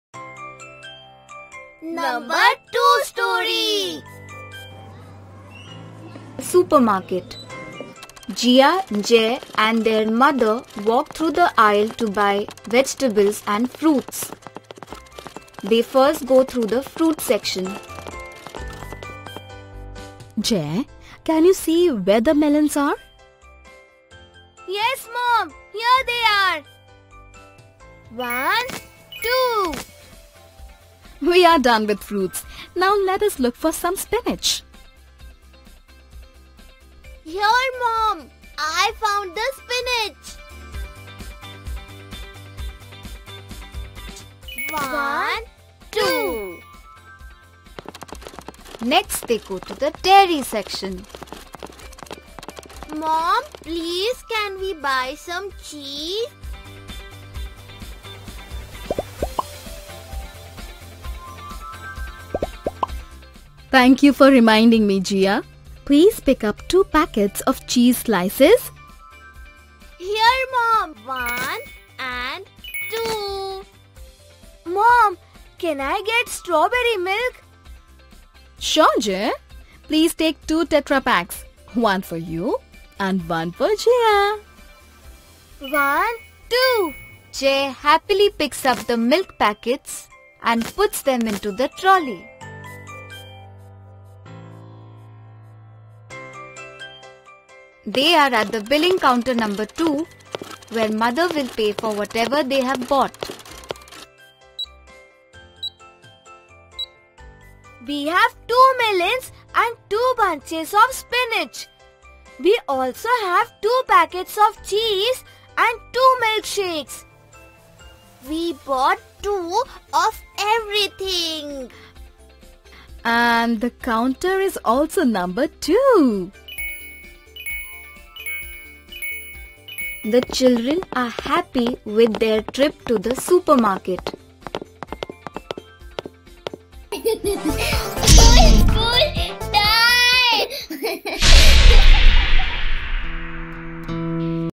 Number Story | #2 | Educational Story about Number 2 by Old School Tie
Old school tie is everything children want their school to be and what better way to do that than making a babies time with us fun and musical? We bring to you the most popular Educational Story about Number 2 in this cartoon video just for kids!